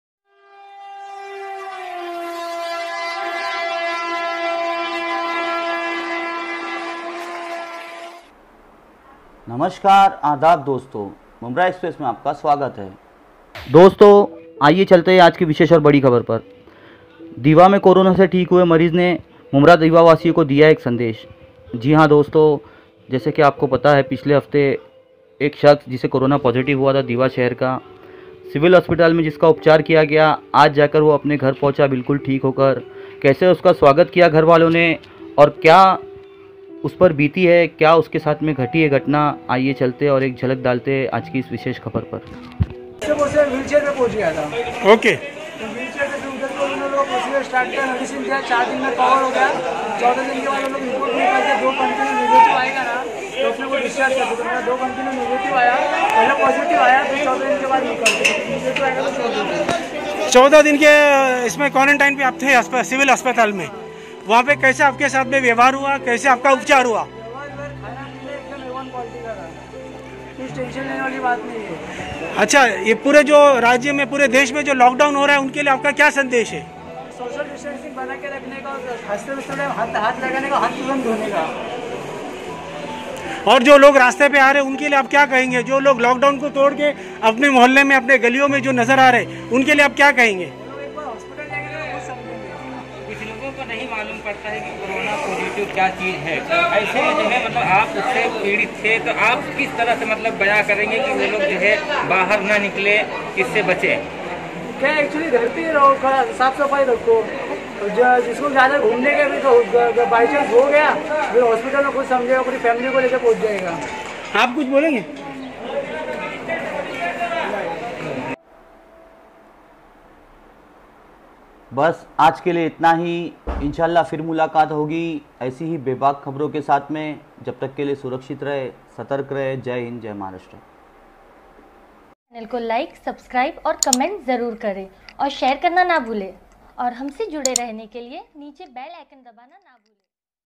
CORONA Mariz Ne Theek Hokar Kya Kaha?|Me Tv
Dosto.
Janiye Kya Paigaam Diya Hai Is Shakhs Ne CORONA Ki Bimari Se Theek Hokar,Munbra-Diva Ke Logo Ko?

Produced by : Mumbra eXpress News
Edited by       : Rafshan Pirani
On screen      : Rafshan Pirani
Alert viewer :